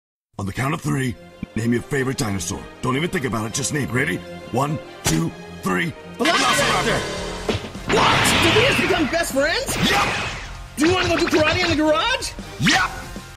Did Goku & Vegeta just become Dragonball BestieZ?!
Some years ago, to promote the North American release of DBZ Resurrection F, Christopher Sabat brought on Chris Ayres (rip) and Sean Schemmel to dub iconic movie scenes in their characters' voices. @SmallButera made an animation using audio of the first segment Schemmel and Sabat voiced, and then I added sound effects and music on top of it.